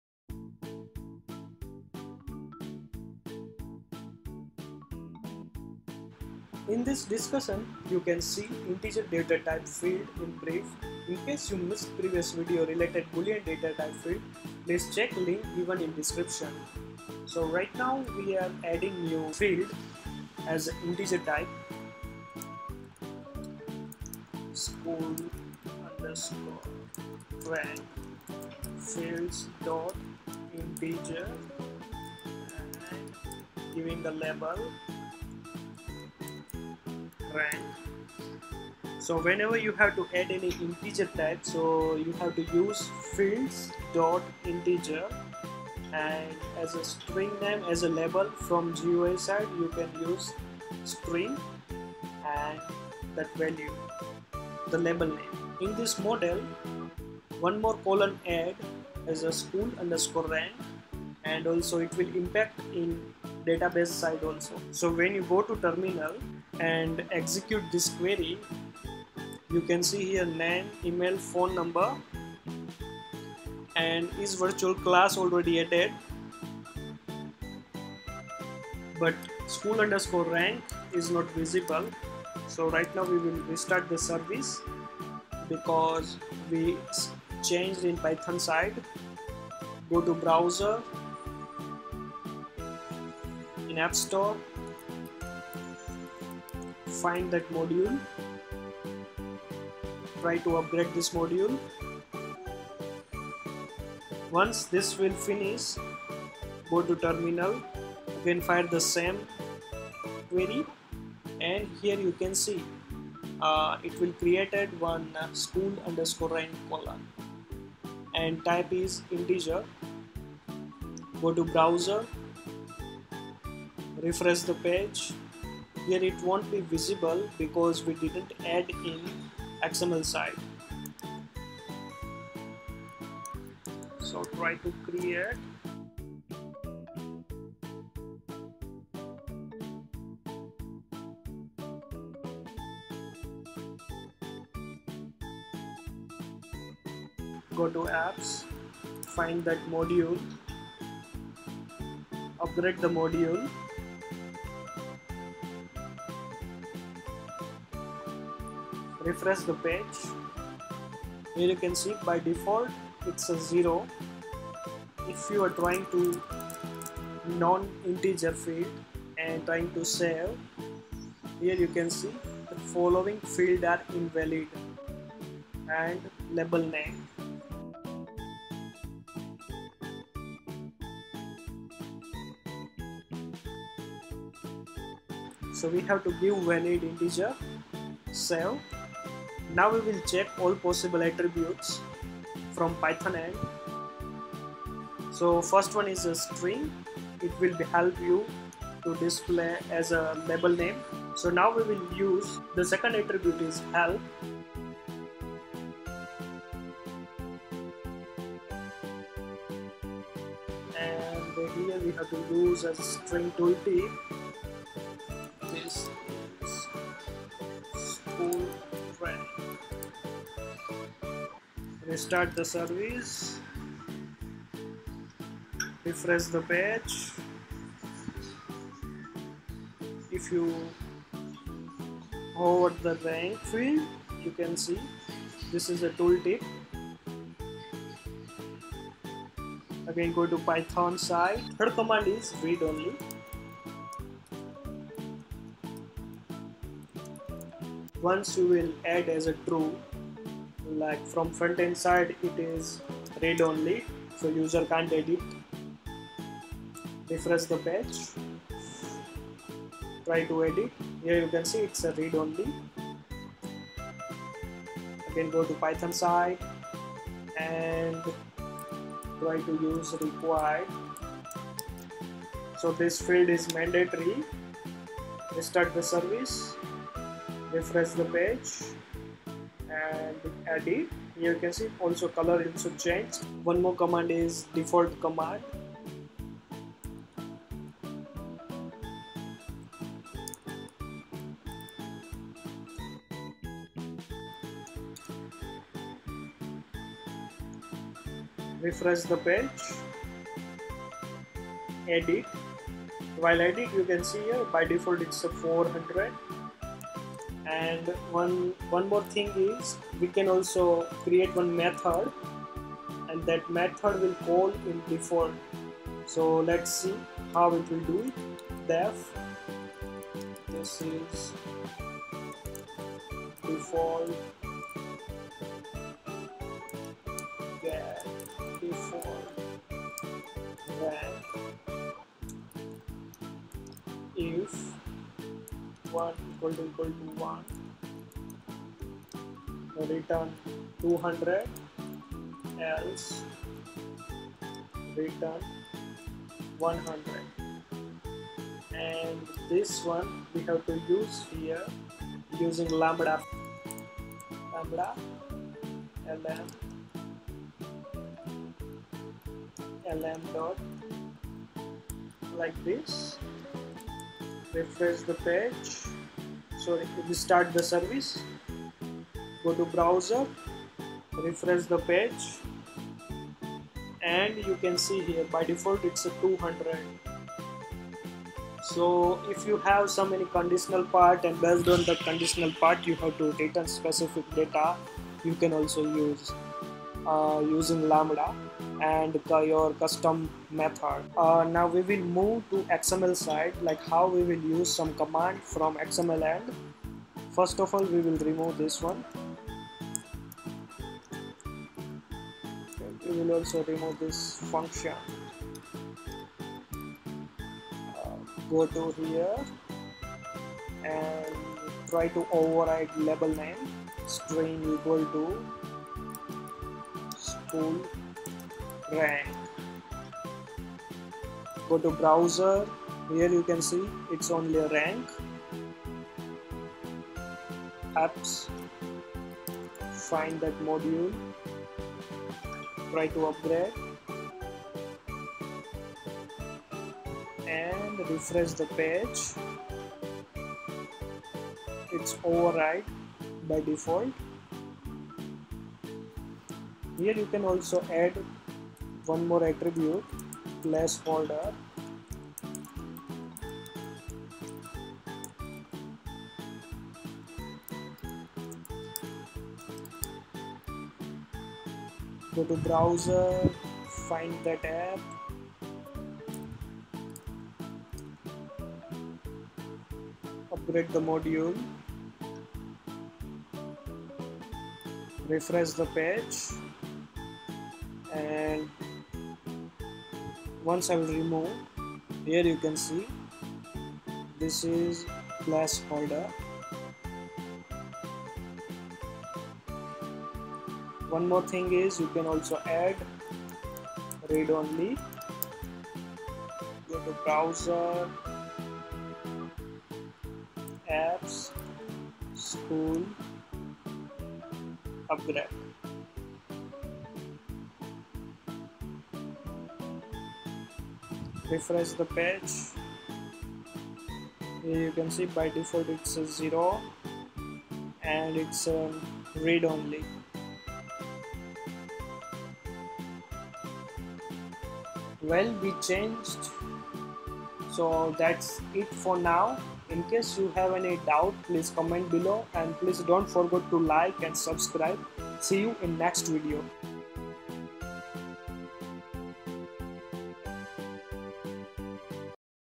How To Create Integer Field | Datatype In Odoo | Supported Attributes | Odoo Fields Tutorial
How to create integer field in Odoo, And supported attributes for int field. Integer fields and its parameter we will see in depth. Odoo supported many types of fields to store into database and Here in this discussion you will get idea about how to create/add Integer field in custom model or existing model in Odoo.